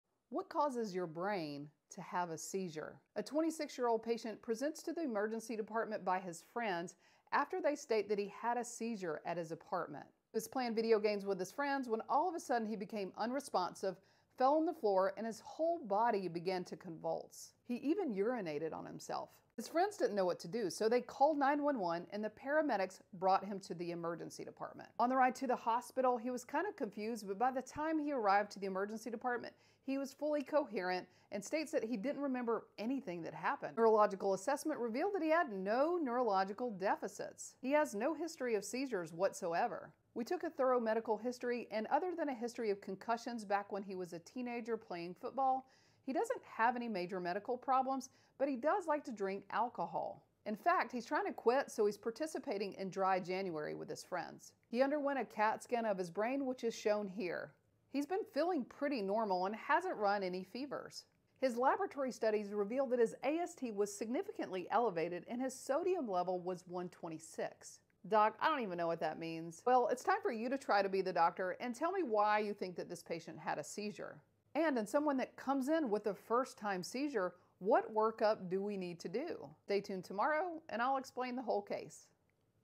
Case study 162 – new onset seizure SundayCaseStudy Ladyspinedoc find out the answer tomorrow ￼
𝐂𝐚𝐬𝐞 𝐬𝐭𝐮𝐝𝐲 𝟏𝟔𝟐: 𝘍𝘪𝘳𝘴𝘵 𝘛𝘪𝘮𝘦 𝘚𝘦𝘪𝘻𝘶𝘳𝘦 ⁣⁣
A 26-year-old man presents to the emergency department with new onset seizure. His history is presented along with his imaging a laboratory study results. What do you think caused the seizure?